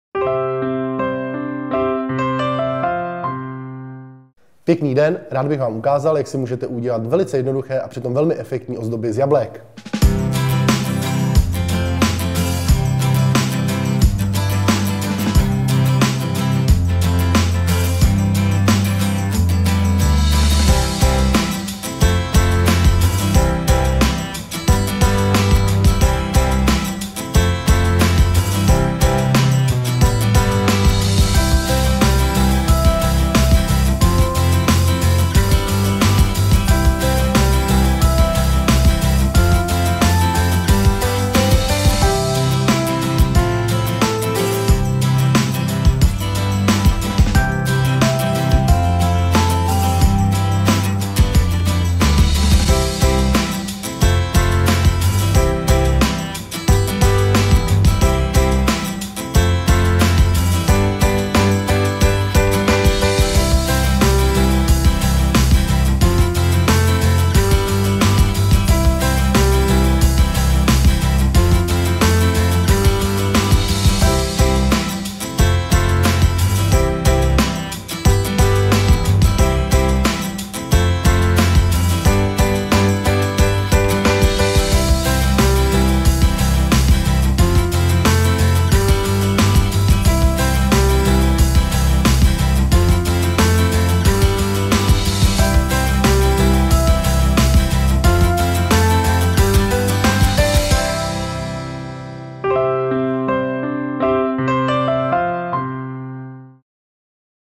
Tesco recepty | Umění carvingu | 2 tipy na ozdobná jablka
Inspirujte se, jak si i vy doma můžete vykrájet jablíčka - třeba na oslavu nebo party! Jan Hajný vám ukáže 2 skvělé tipy... - čerstvá inspirace.